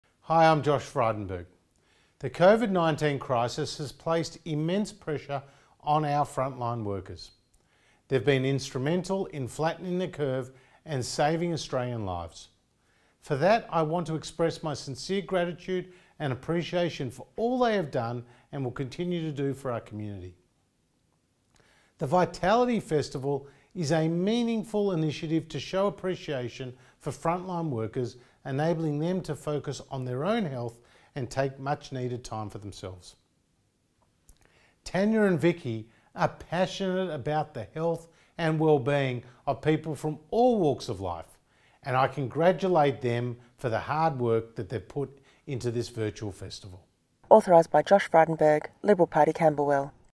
Josh Frydenberg's endorsement Video for VITAlity festival
In this video Josh Frydenberg, Treasurer of Australia shares his message of encouragement for the VITAlity Festival™️ and why this is an important event to help our forgotten heroes.